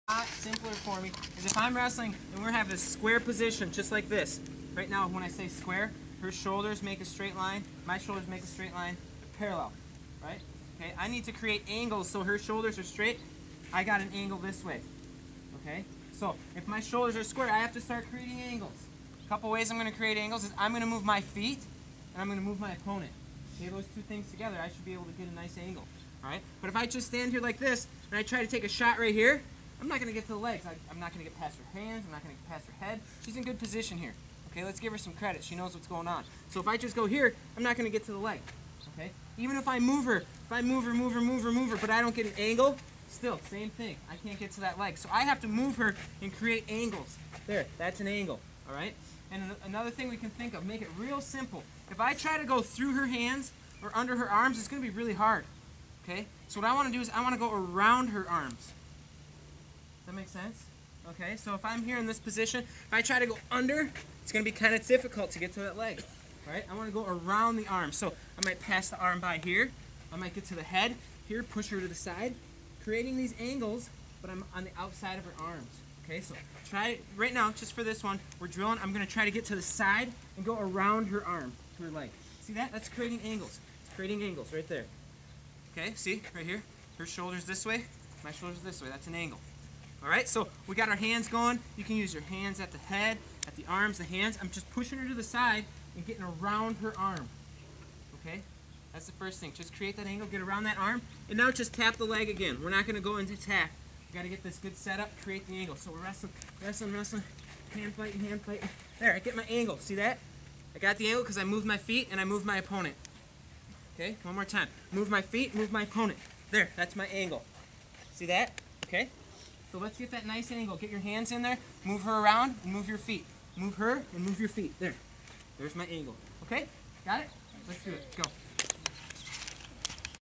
Creating Angles - Kevin Black Technique
Kevin Black at the US Olympic Development Camp in Chula Vista, CA on October 29, 2009.  Kevin demonstrates creating angles prior to leg attacks.  Using hands and moving feet is how one creates angles from the standing (neutral) position in wrestling.